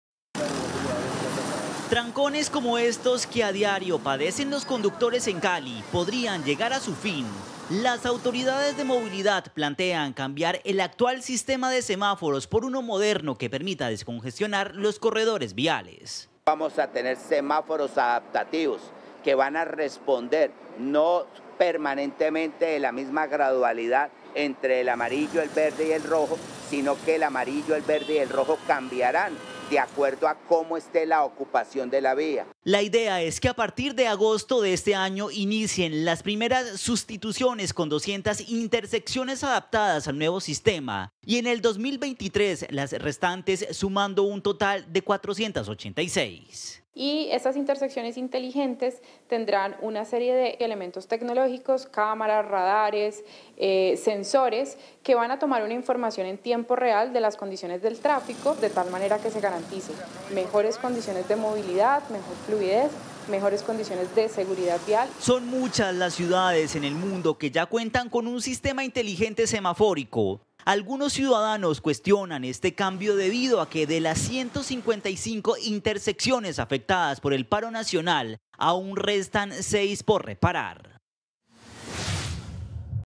Nueva red de semáforos inteligentes le costaría $55 mil millones a Cali
Cali | La ciudad busca sustituir la actual red semafórica por un sistema inteligente para controlar los tiempos y reducir los trancones, que constaría $55 mil millones de pesos.

Actualmente, el proyecto se encuentra en el proceso precontractual de obra civil y adquisición de los elementos tecnológicos para iniciar con la ejecución a partir del segundo semestre del año.